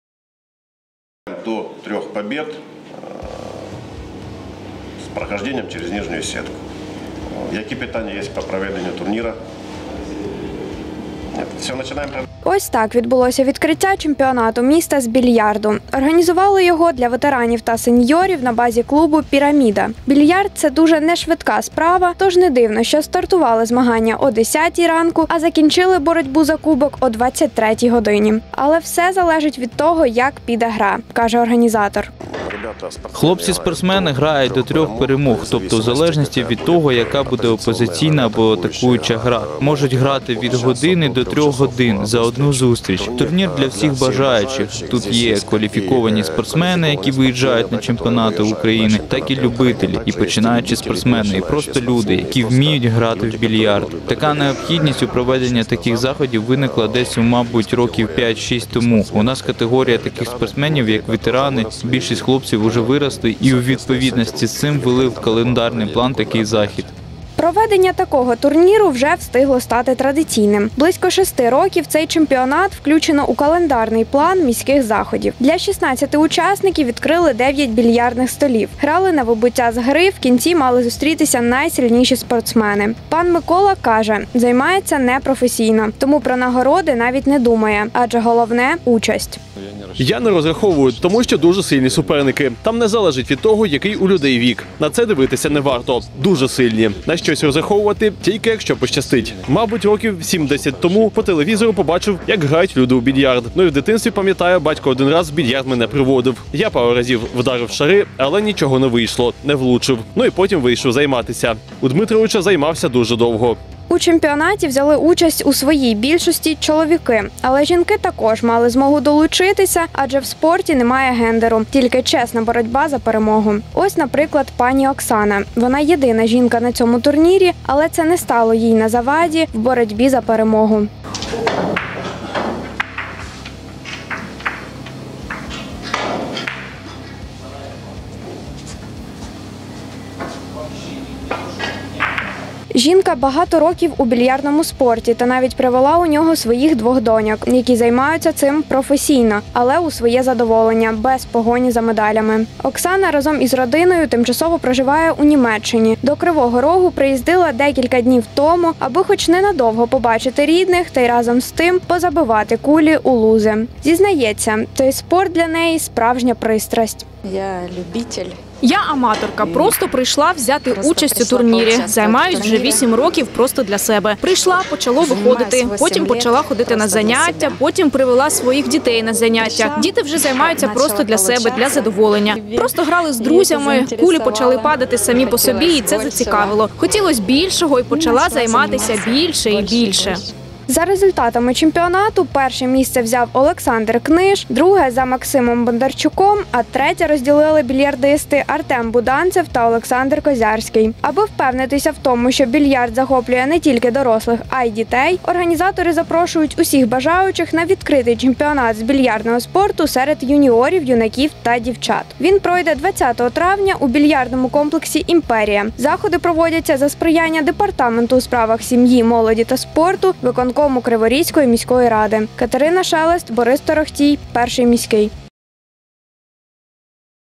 У Кривому Розі провели відкритий чемпіонат із більярду
Для ветеранів та сеньйорів. У більярдному комплексі «Піраміда» пройшов Відкритий чемпіонат Кривого Рогу з більярдного спорту серед. У змаганнях взяли участь всі бажаючі, але віком старше 40 років. Всього за Кубок боролись 16 учасників, а як це було бачила наша Катерина Шелест.